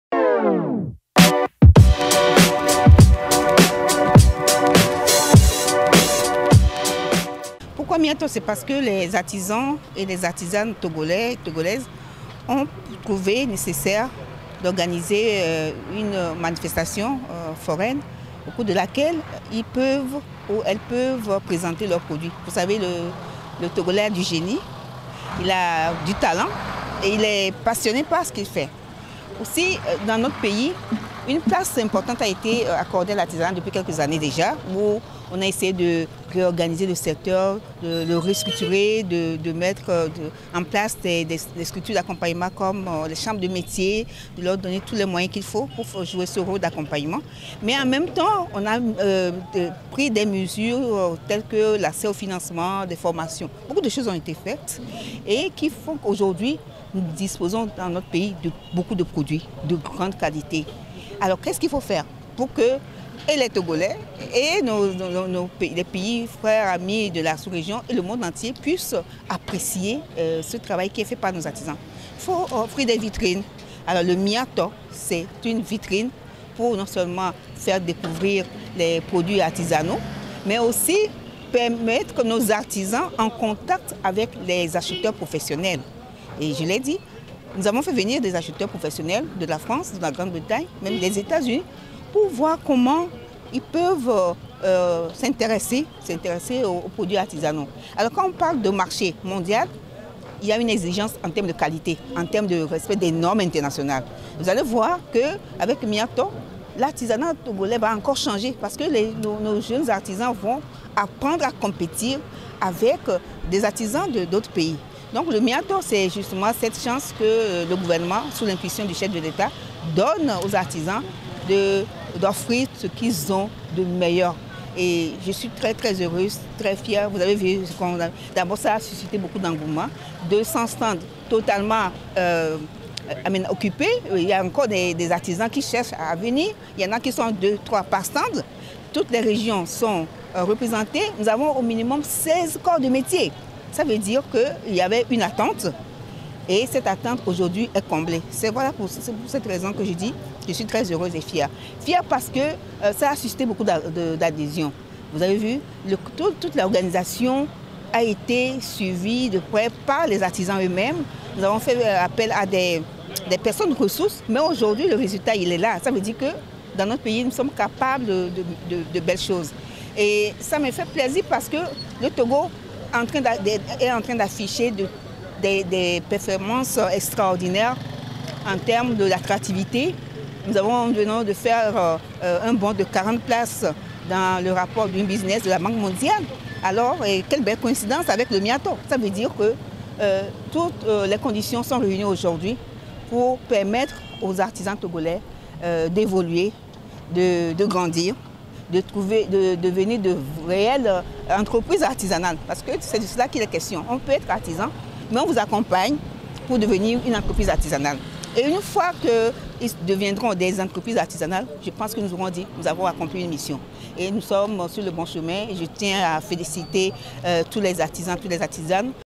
Madame le ministre, pourquoi le MIATO ?
Après la cérémonie d'ouverture du marché international de l'artisanat du Togo le 28 Octobre dernier, le ministre de l'artisanat du Togo Victoire Tomegah Dogbe a accordé une interview à direct7 à propos du  MIATO.
Suivez plutôt!